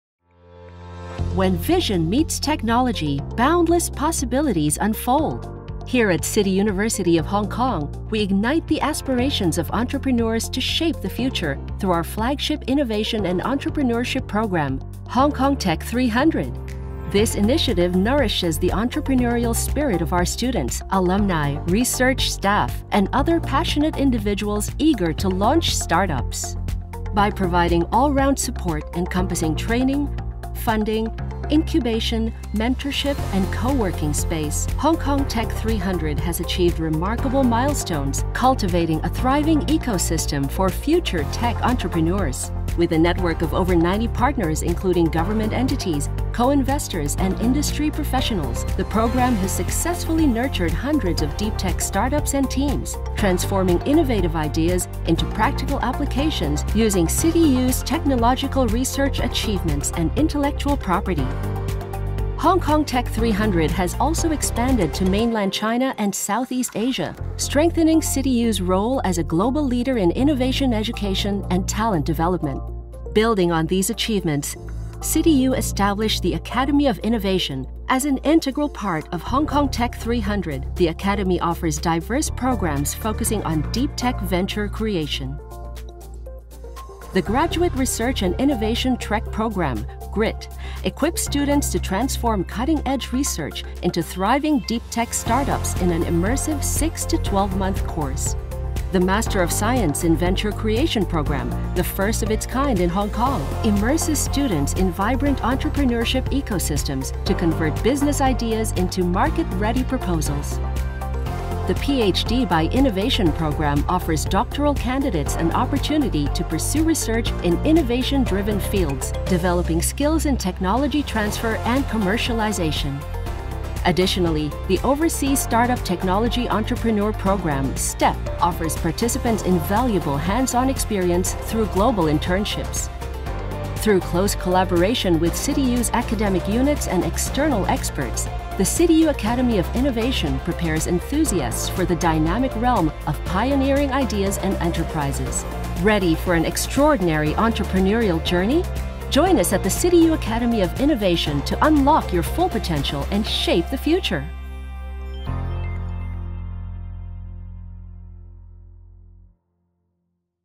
【城大創新學院助有志者創業啓航，塑造未來! /Ignite Your Innovation Journey: Introducing the CityU Academy of Innovation!】
城大於創新及創業教育方面表現卓越，新成立的城大創新學院將有助大學進一步在創新教育及人才培育方面，成為全球領先的高等學府。

作為城大旗艦創新創業計劃 HK Tech 300 重要的一環，城大創新學院提供一系列適合本科、碩士和博士生修讀的創新創業學術課程，培育年輕的科研人員成為創業家，孵化深科技初創企業，促進產學研合作，以發展國際領先的創科生態系統。課程包括：哲學博士學位 ( 創新創業)、理學碩士學位 ( 創新創業)、研究生創新創業啟航課程及海外初創企業實習計劃。

As a leading global university renowned for its commitment to innovation education and entrepreneurship development, CityU takes pride in establishing the CityU Academy of Innovation (the “Academy”). This pioneering initiative aims to maintain the University’s position as a leader in the field and to pursue unparalleled excellence in innovation education, talent cultivation and entrepreneurship development.

As an integral part of our flagship innovation and entrepreneurship programme – HK Tech 300, the Academy  offers a series of innovative academic programmes at PhD, Master of Science (MSc) and undergraduate (UG) programmes, focusing on deep-tech venture creation, to develop a leading international ecosystem that nurtures young entrepreneurs, incubates deep tech start-ups and promotes university-industry collaboration. The programmes include: PhD by Innovation, Master of Science in Venture Creation, the Graduate Research and Innovation Trek Programme (GRIT), and the Overseas Start-up Technology Entrepreneur Programme (STEP).